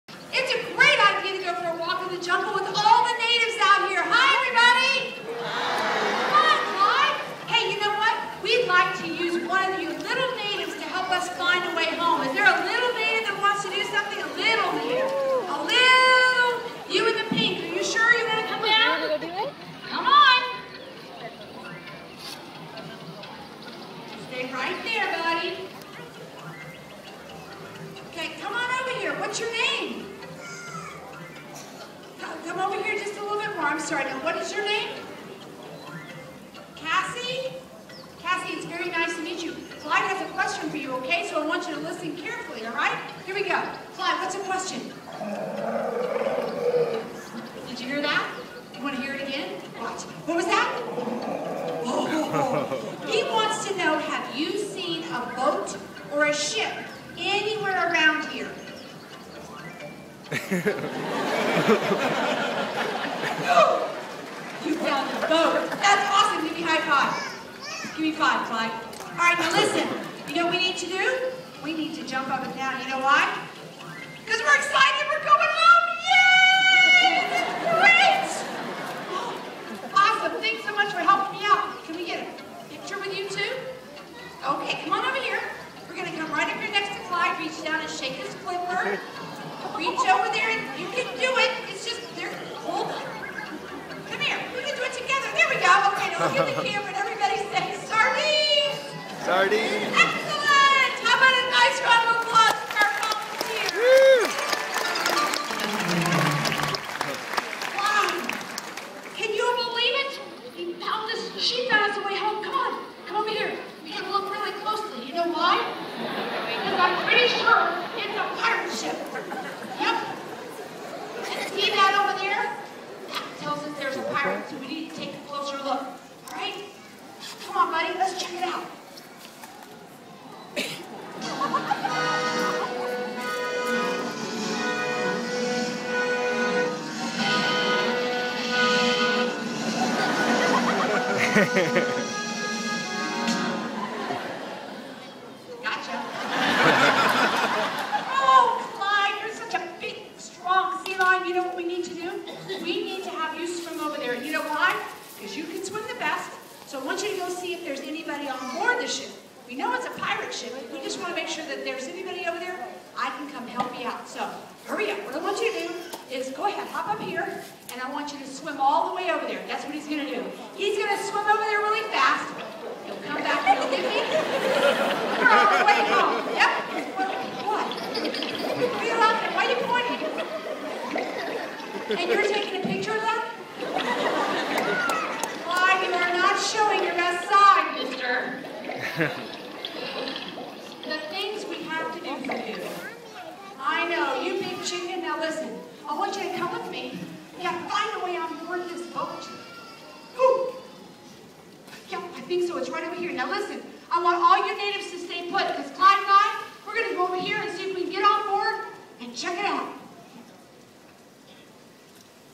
Cassidy Meets Clyde The Sea Lion
Friday, October 21, 2011 Cassidy meets Clyde the Sea Lion from Sea World in Orlando.  We had a very fun day and give special thanks to Sea World and it's staff for giving Cassidy the opportunity to shake a sea lions "hand"!